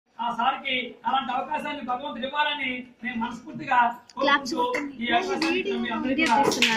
Service at PALLIPADU DIET SCHOOL(1)
13.2.19 ఉదయం 10 గం లకు పల్లిపాడు లోని MPPP పాఠశాల పిల్లల కు స్టీల్ ప్లేట్స్ ,గ్లాసుల ,స్పూన్, కప్ ల పంపిణీ  చేసినాము. దాత.మోడెమ్ లక్ష్మి రాజ్యం గారి జ్ఞాపకార్ధం వారి కుమారుడు లయన్ మోడెమ్ సూర్య ప్రకాష్ గారు..PST స్